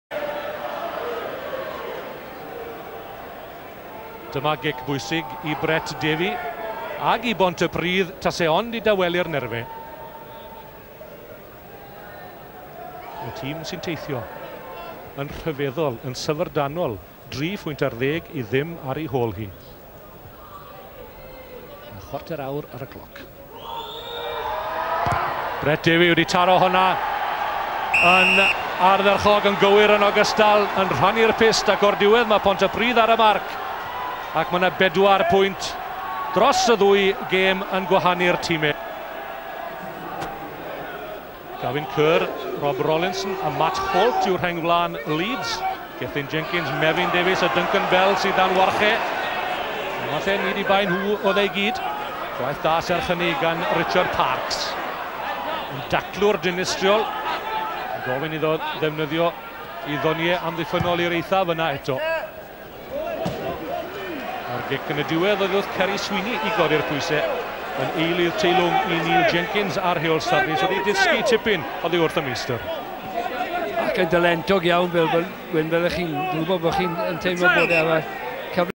05 Leeds V Pontypridd
Ponty's 3rd Ginger Monster kicks a penalty - Brett Davey